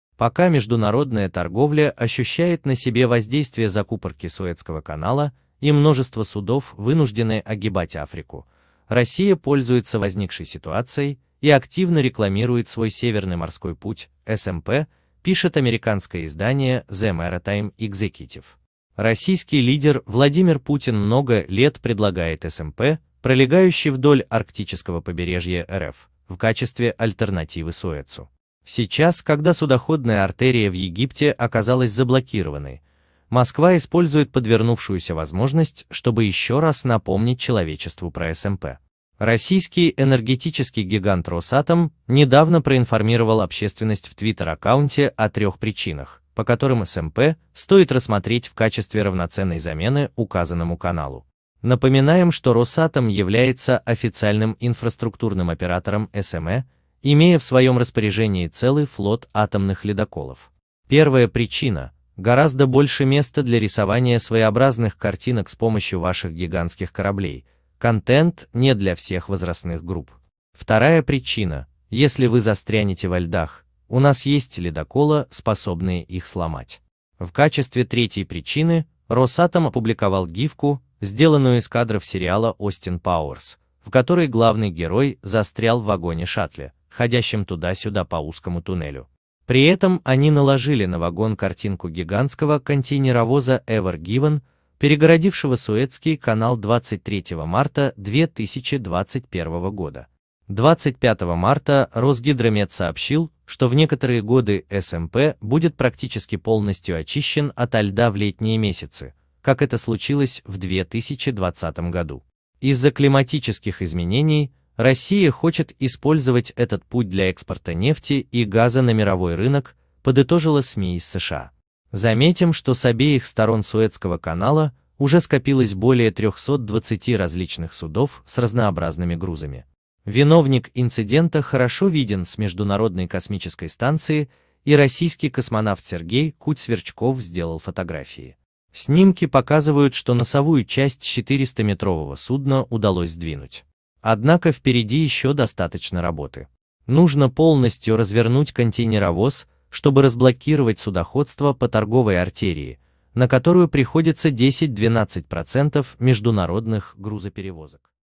СМИ США: Россия пользуется проблемой в Суэцком канале, чтобы разрекламировать Северный морской путь
Пока международная торговля ощущает на себе воздействие «закупорки» Суэцкого канала и множество судов вынуждены огибать Африку, Россия пользуется возникшей ситуацией и активно рекламирует свой Северный морской путь (СМП), пишет американское издание The Maritime Executive.

Российский лидер Владимир Путин много лет предлагает СМП, пролегающий вдоль арктического побережья РФ, в качестве альтернативы Суэцу. Сейчас, когда судоходная артерия в Египте оказалась заблокированной, Москва использует подвернувшуюся возможность, чтобы ещё раз напомнить человечеству про СМП.

Российский энергетический гигант «Росатом» недавно проинформировал общественность в Twitter-аккаунте о трех причинах, по которым СМП стоит рассмотреть в качестве равноценной замены указанному каналу. Напоминаем, что «Росатом» является официальным инфраструктурным оператором СПМ, имея в своем распоряжении целый флот атомных ледоколов.

Первая причина – «гораздо больше места для рисования своеобразных картинок с помощью ваших гигантских кораблей» (контент не для всех возрастных групп).

Вторая причина – «если вы застрянете во льдах, у нас есть ледоколы, способные их сломать».

В качестве третьей причины, «Росатом» опубликовал гифку, сделанную из кадров сериала «Остин Пауэрс», в которой главный герой застрял в вагоне-шаттле, ходящем туда-сюда по узкому туннелю. При этом они наложили на вагон картинку гигантского контейнеровоза Ever Given, перегородившего Суэцкий канал 23 марта 2021 года.

25 марта Росгидромет сообщил, что в некоторые годы СМП будет практически полностью очищен ото льда в летние месяцы, как это случилось в 2020 году. Из-за климатических изменений Россия хочет использовать этот путь для экспорта нефти и газа на мировой рынок, подытожило СМИ из США.

Заметим, что с обеих сторон Суэцкого канала уже скопилось более 320 различных судов с разнообразными грузами. Виновник инцидента хорошо виден с Международной космической станции и российский космонавт Сергей Кудь-Сверчков сделал фотографии.

Снимки показывают, что носовую часть 400-метр+ового судна удалось сдвинуть. Однако впереди ещё достаточно работы. Нужно полностью развернуть контейнеровоз, чтобы разблокировать судоходство по торговой артерии, на которую приходится 10-12% международных грузоперевозок.